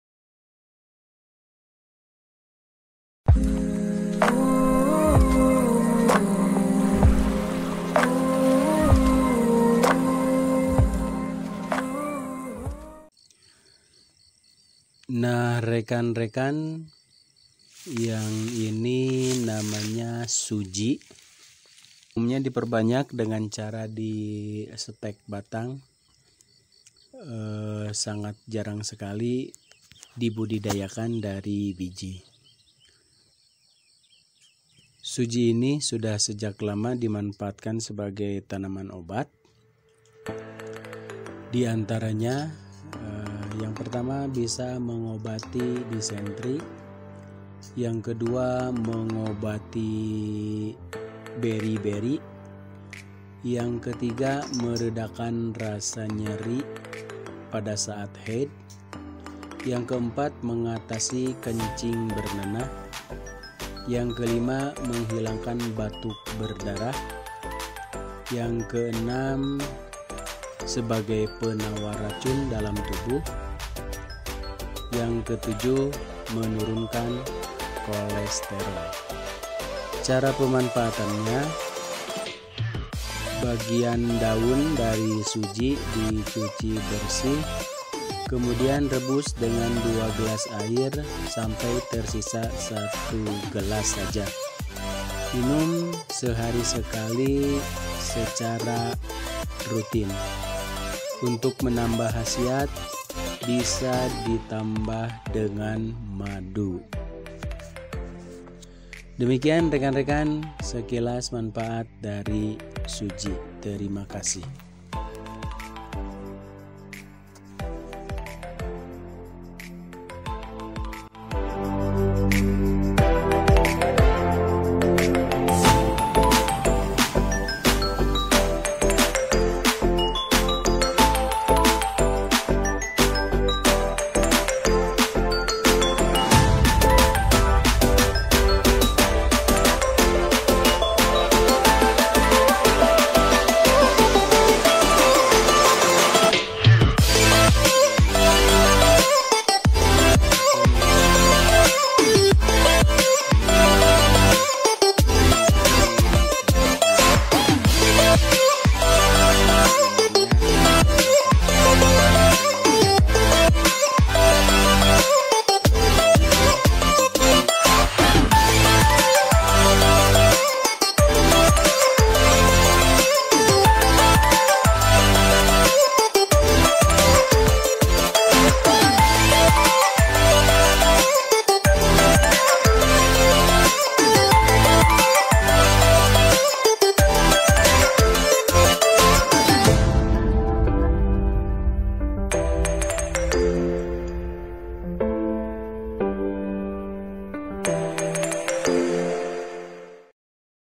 tanaman suji ternyata bisa sembuhkan berbagai penyakit
suji merupakan tanaman perdu dari jenis bambu bambuan